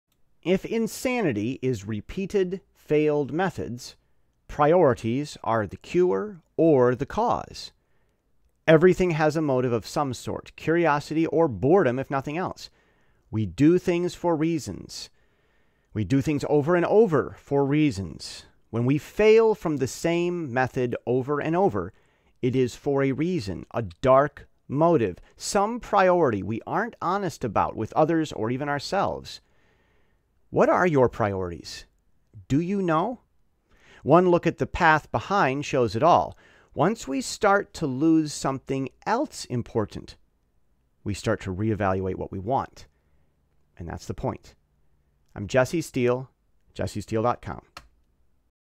Priorities Guide Change | The Point
If insanity is repeated, failed methods, priorities are the cure or the cause. Everything has a motive of some sort, curiosity or boredom if nothing else. We do things for reasons. We do things over and over for reasons. When we fail from the same method over and over, it is for a reason—a dark motive—some priority we aren't honest about, with others or even ourselves.

What are your priorities? Do you know? One good look at the path behind shows it all. Once we start to lose something else important, we start to reevaluate what we want.